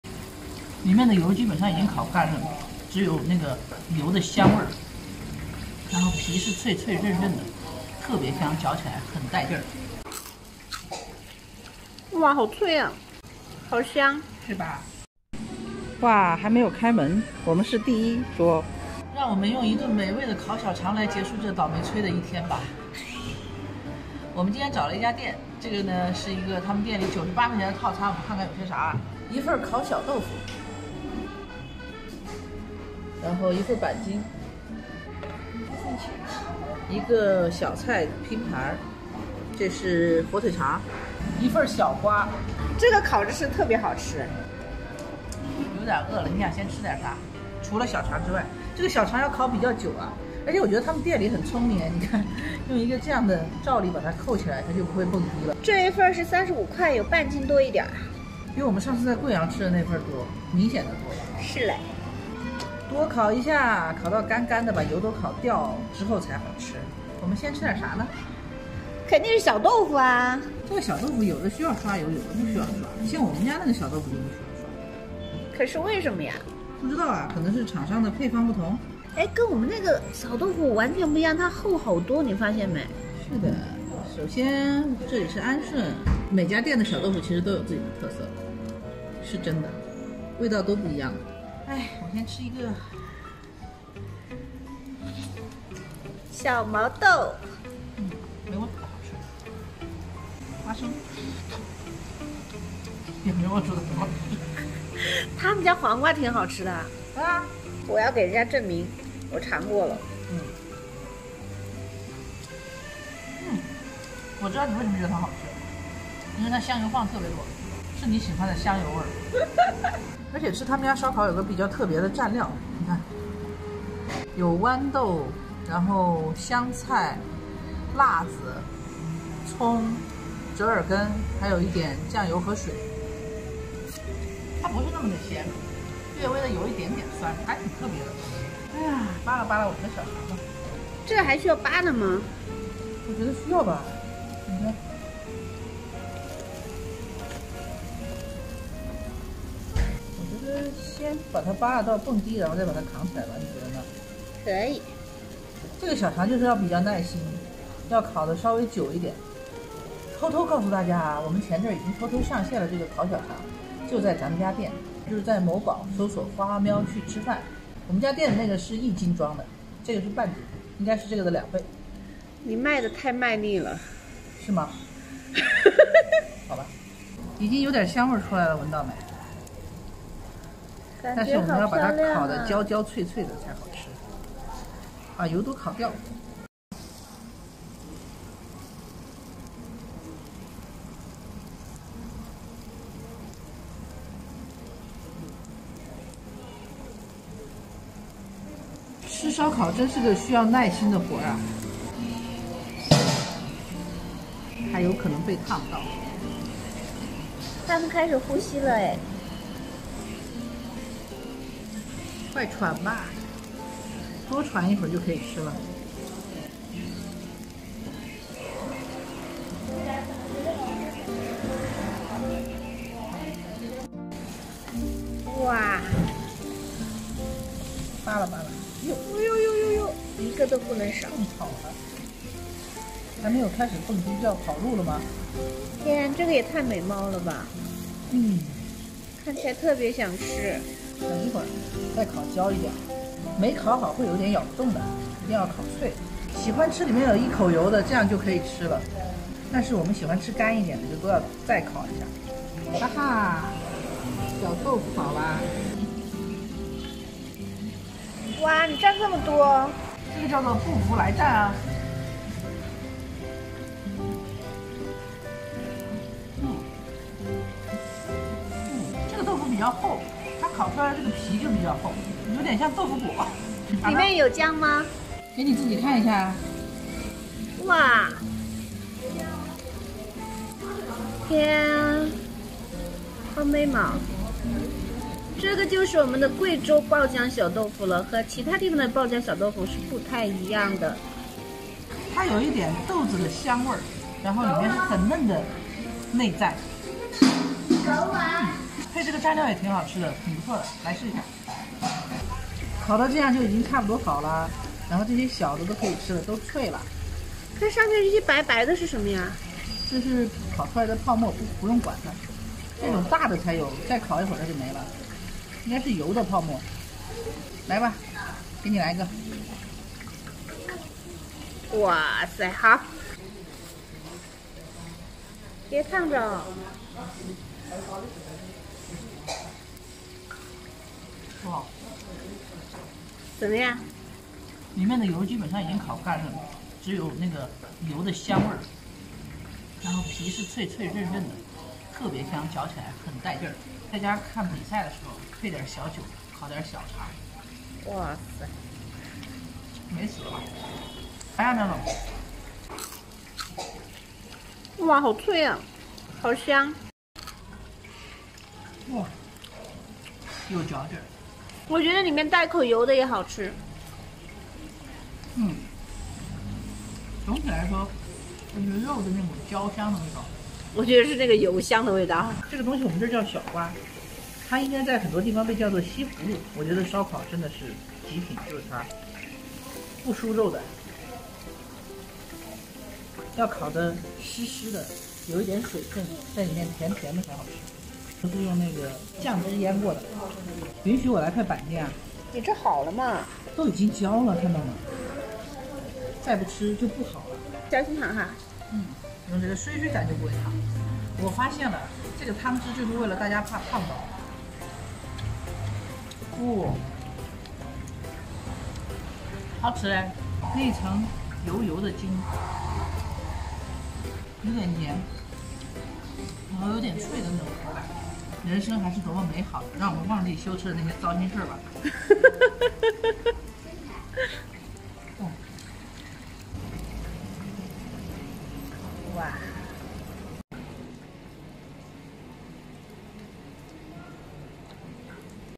焦香金黄的烤小肠滋滋冒油，瞬间治愈被生活暴击的我，吃肉好快落【花喵去吃饭】
今天终于到安顺来吃烤小肠了，跟贵阳不一样，这边的烤小肠，量！好！大！而且不知道是不是心理作用，感觉更好吃咧